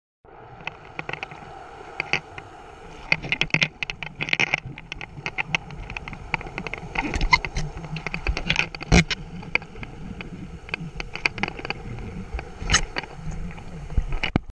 WHALESHARK TOUR ISLA MUJERES - MEXICO WHALE SHARK
¡Enjoy the Experience!
Snorkeling tour swimming with whale shark in México.
THE BIGGEST FISH IN THE OCEAN.

VIDEO TAKEN ON JULY 20TH

WE GO OUT FROM: PLAYA ,COZUMEL, TULUM, ISLA MUJERES, CANCUN AND HOLBOX